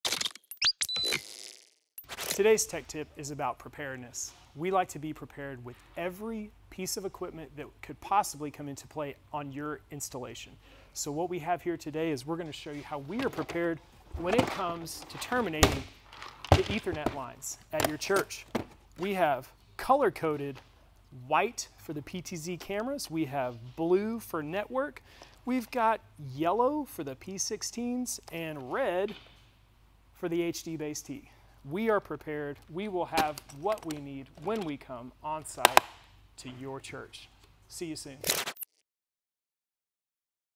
How we organize our ethernet terminations
In this weeks Tech Tip, Jonathan shows how the SIC team organizes and uses RJ45 boots for various ethernet terminations.